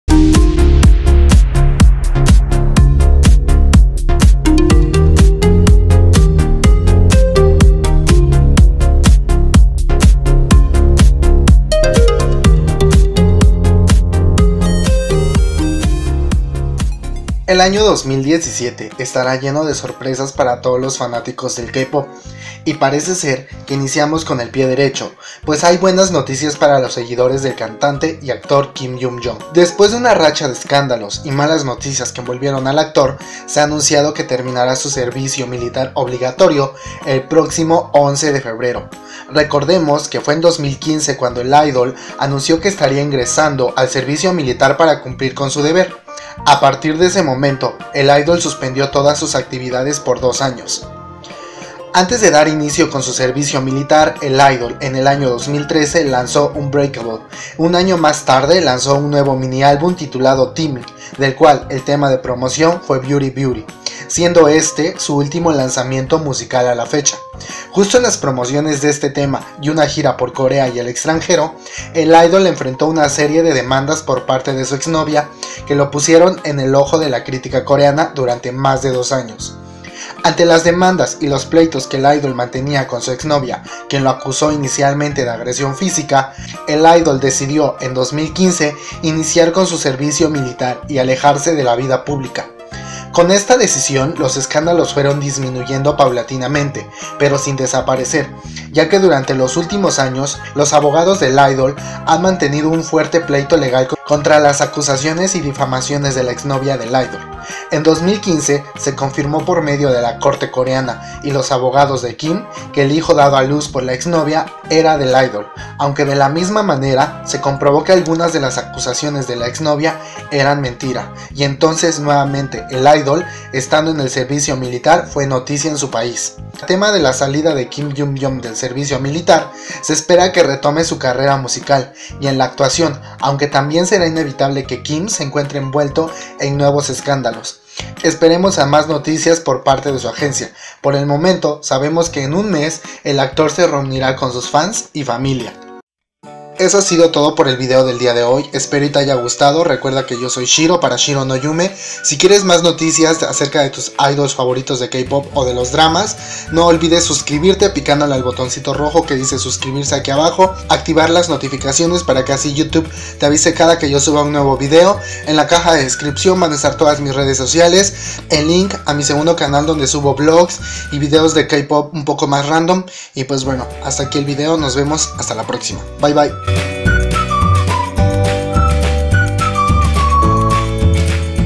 Kim Hyun Joong Vuelve del Servicio Militar | Noticias de Kim Hyun Joong | Shiro No Yume
Se ha anunciado que el cantante y actor KIM HYUN JOONG terminara su servicio militar el día 11 de Febrero de 2017, mira el vídeo para enterarte de los detalles.